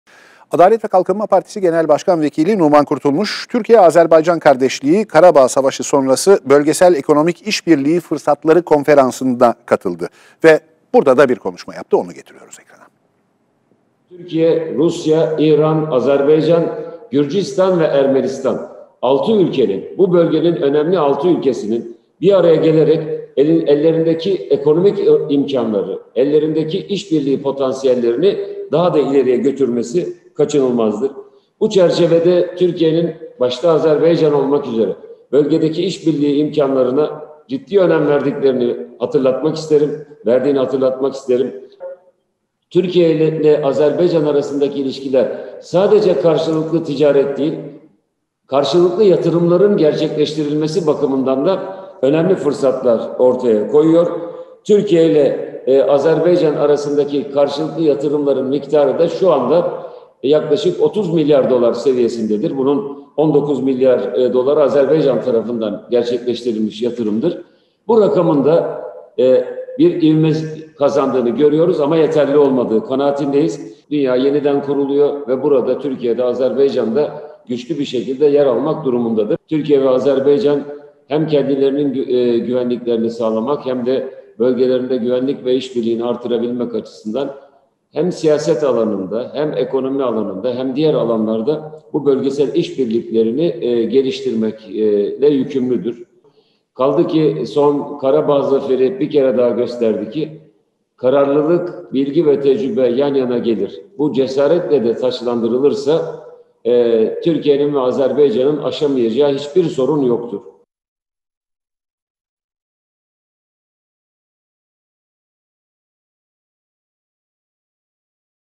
Numan Kurtulmuş: Yeniden Kurulan Dünyada Türkiye ve Azerbaycan Birlikte Yer Almalı
Numan Kurtulmuş'tan Azerbaycan mesajı